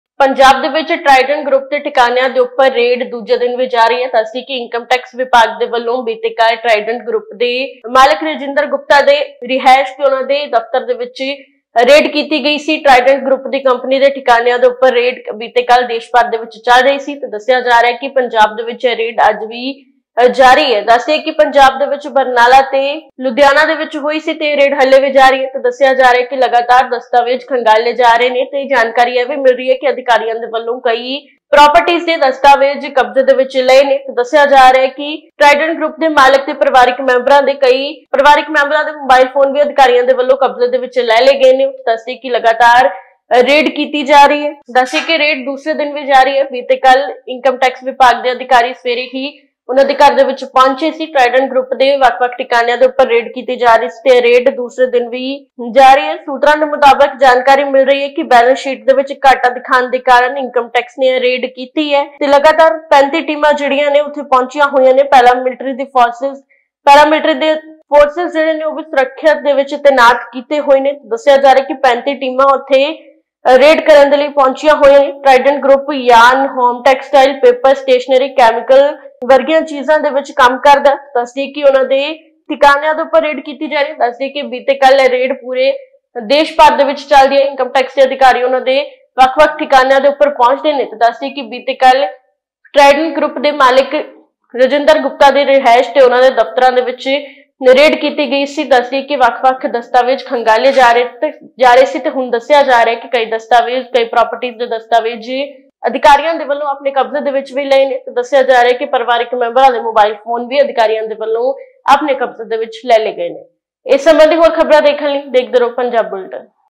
Trident Group ਦੇ ਠਿਕਾਣਿਆਂ ਤੇ ਦੂਜੇ ਦਿਨ ਵੀ Income Tax ਵਿਭਾਗ ਦੀ ਰੇਡ ਜਾਰੀ | Punjabi Bulletin
Trident Group ਦੇ ਠਿਕਾਣਿਆਂ ਤੇ ਦੂਜੇ ਦਿਨ ਵੀ Income Tax ਵਿਭਾਗ ਦੀ ਰੇਡ ਜਾਰੀ

Punjabi Bulletin